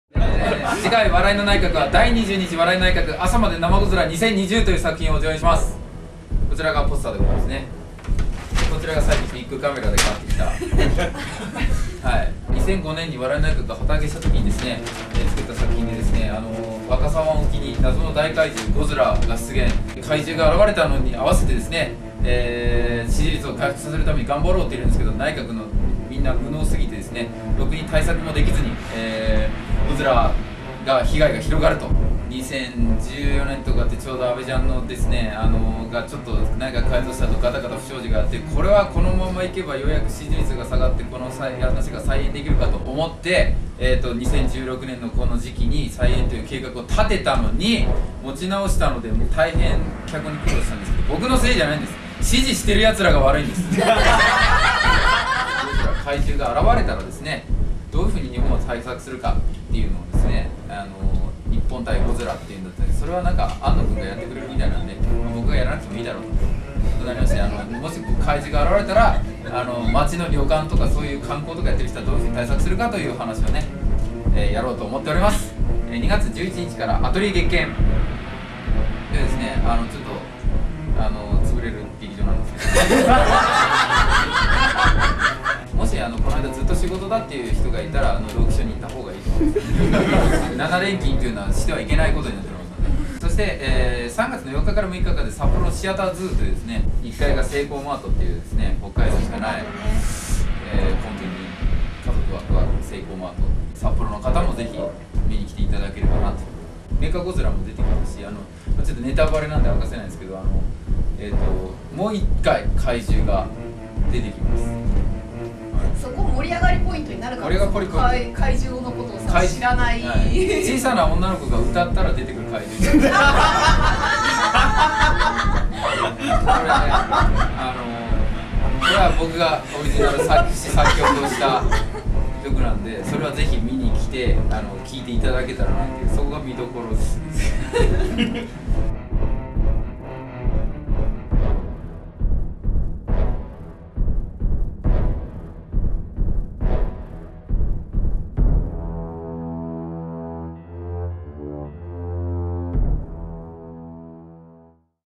【SPCM】笑の内閣『朝まで生ゴヅラ2020』告知
笑の内閣10周年最後の公演は、もし怪獣が現れたらを描くシミュレーションコメディ！
只今、公演初日14日前まで購入可能な先得決済14を発売中！